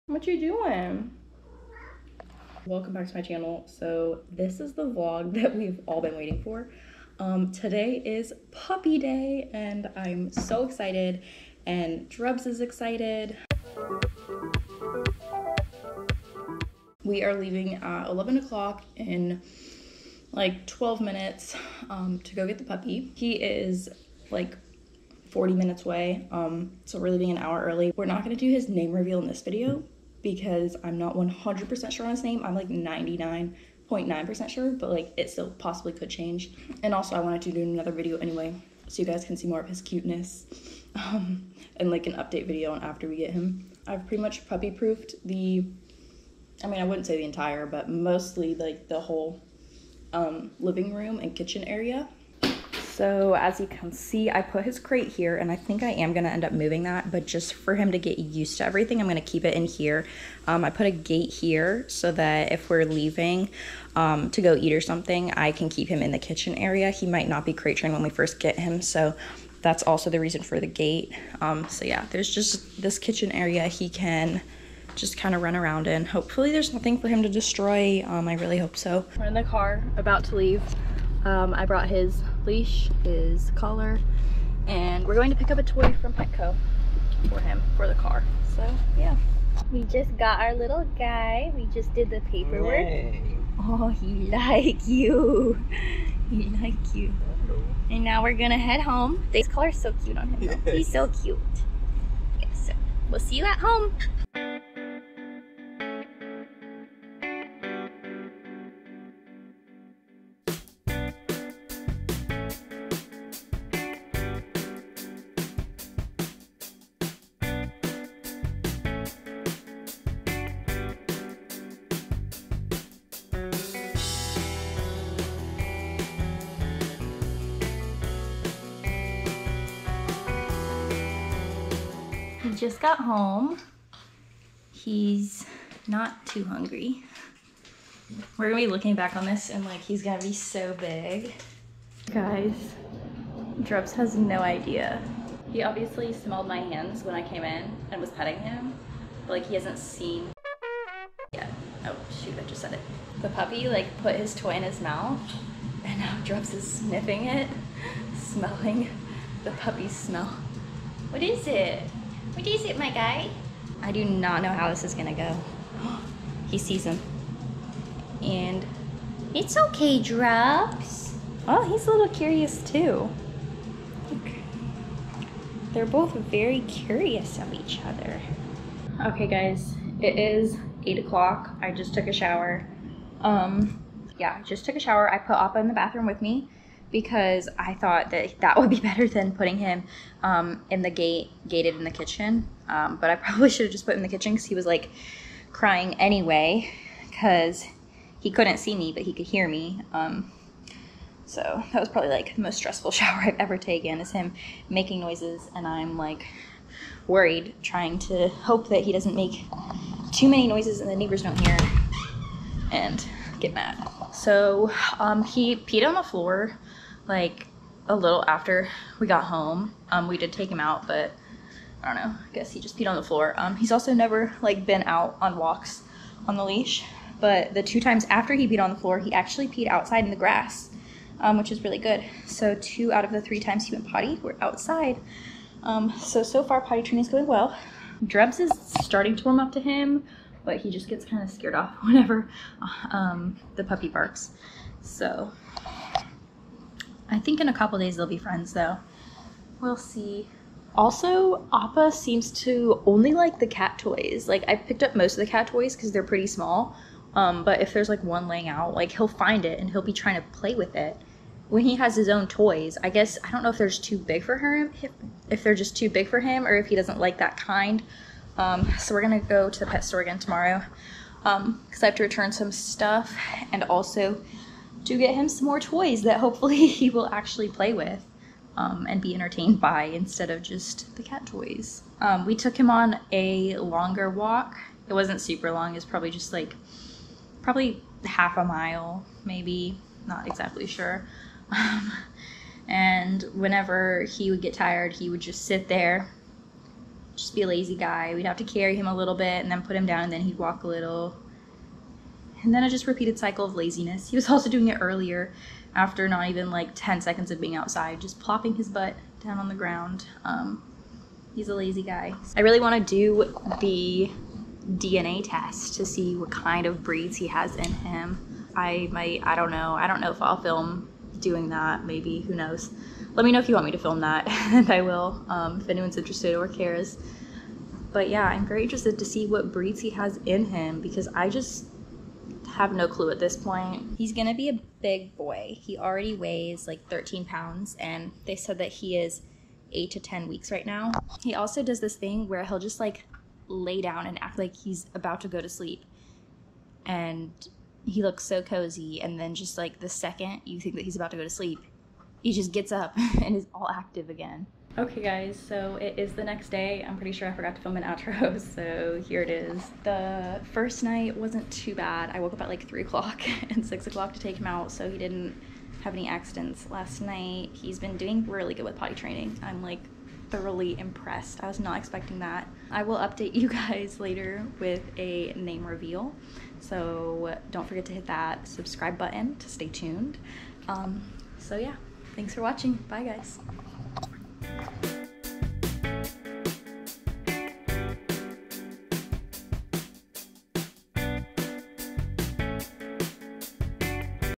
Bringing home our new PUPPY!!!
Getting our puppy and an update on his first day home!

About me:
How old are you? 20
Where are you from? Northern Va / Nc
How many cats do you have? 5 🤪
How many dogs do you have? 1